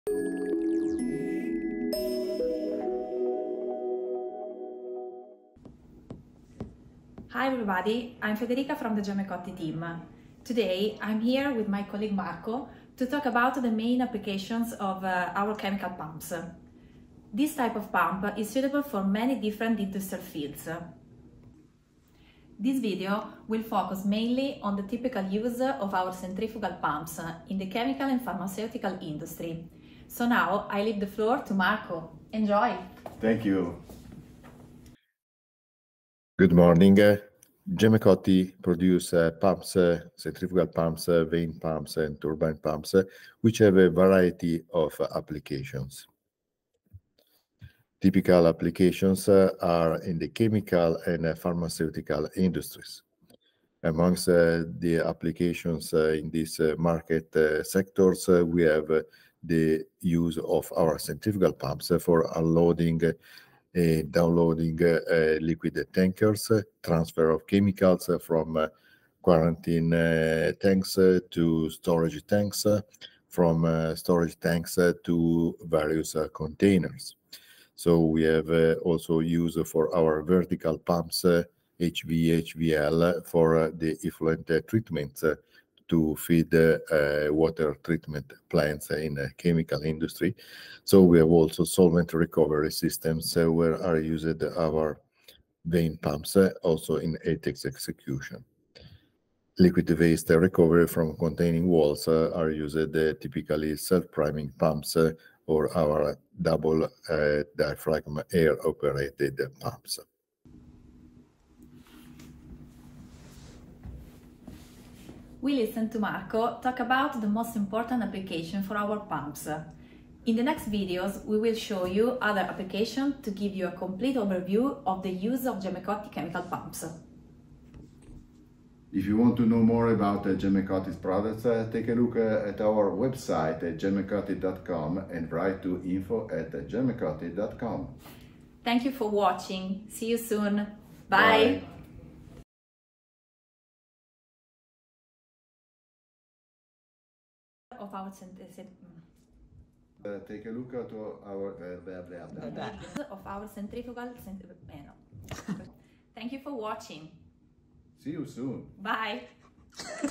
GemmeCotti chemical pumps - main applications
GemmeCotti manufactures chemical pumps suitable for different applications and tailored for various industrial fields. This video will focus mainly on the typical use of GemmeCotti centrifugal pumps in the chemical and pharmaceutical industry. In the next videos, we will show you other applications to give you a complete overview of the use of our chemical pumps suitable for acids and dangerous liquids.
Listen to our colleagues Marco and Federica explain the main industrial applications of our centrifugal pumps.